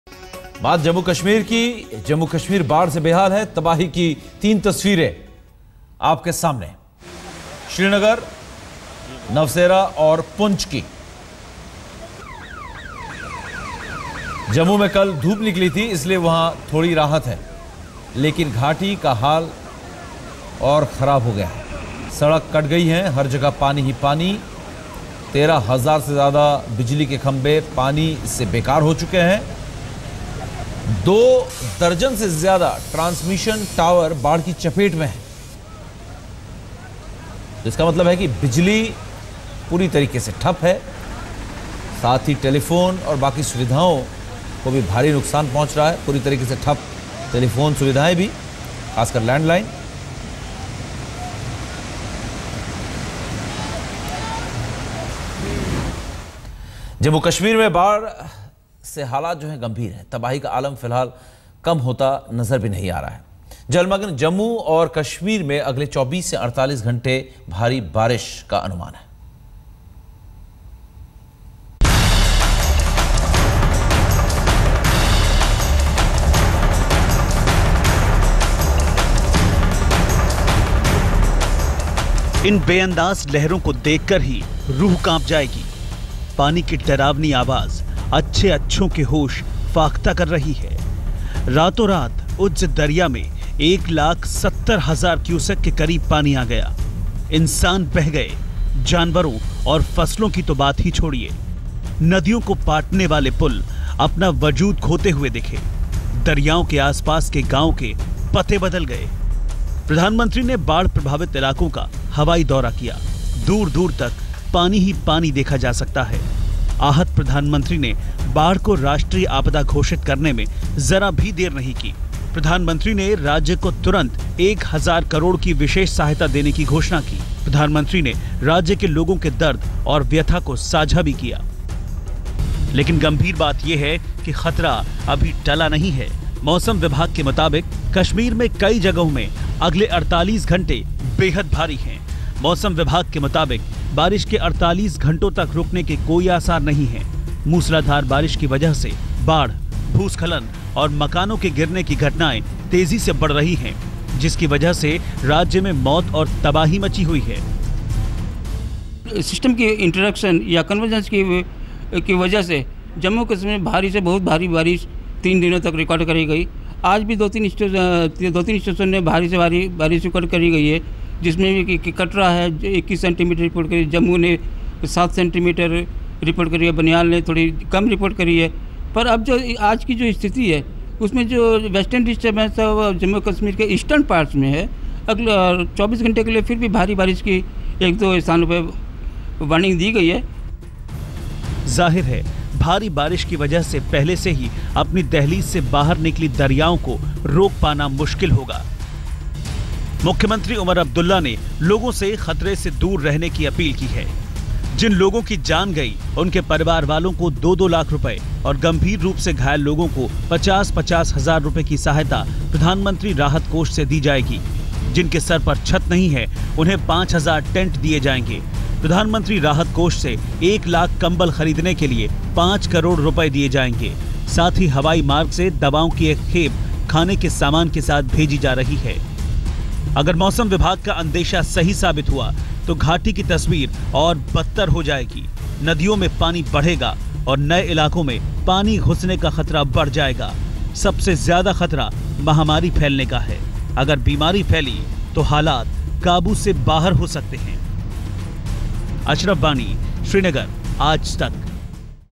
Flood woes: No electricity, phone network in Jammu and Kashmir
Jammu and Kashmir is facing its ultimate calamity with thousands of transmission towers and electric poles damaged due to the floods that have ravaged the state over the last four days. Hence, power supply and phone network is almost nil in the state.